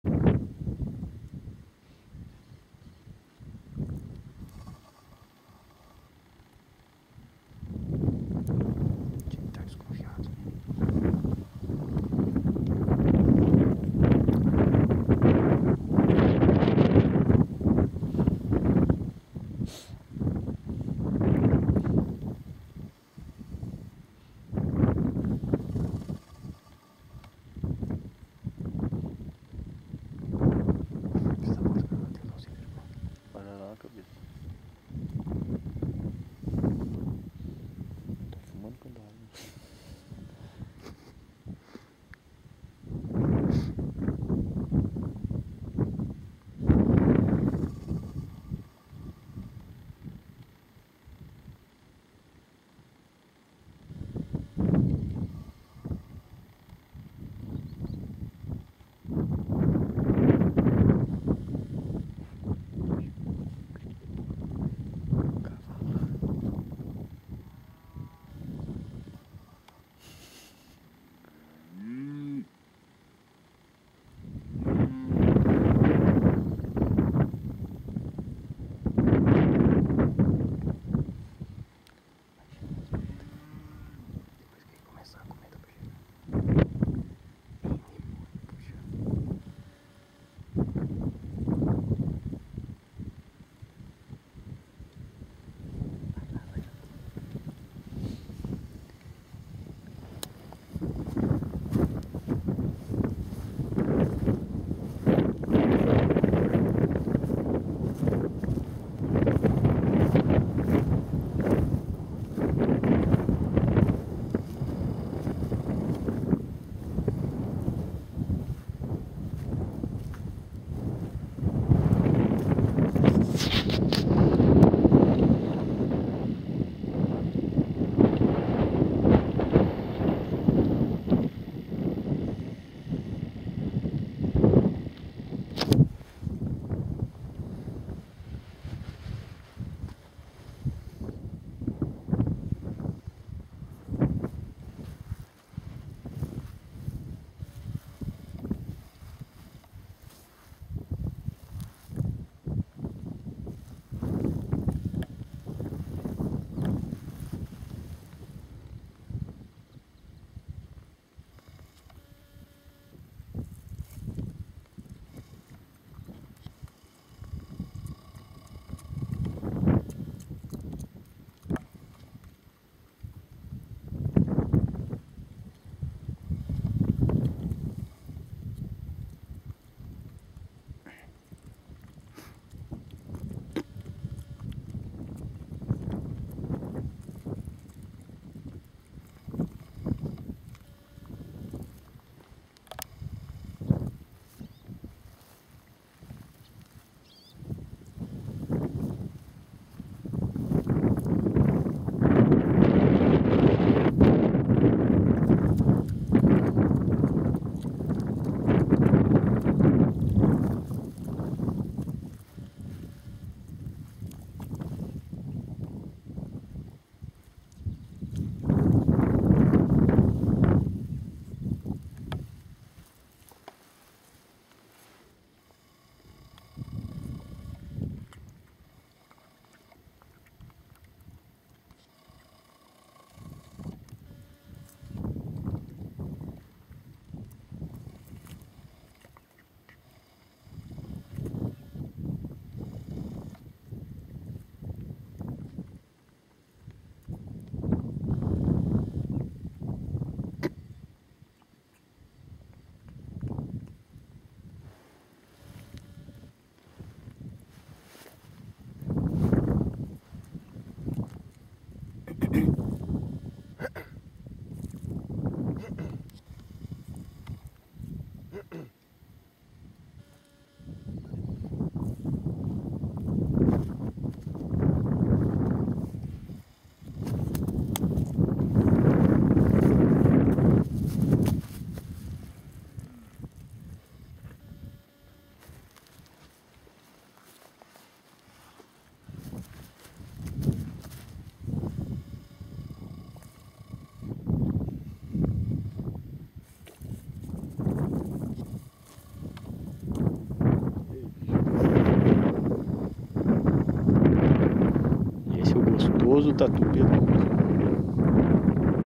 TATU carnívoro
Flagramos um  tatu comendo uma vaca.. !!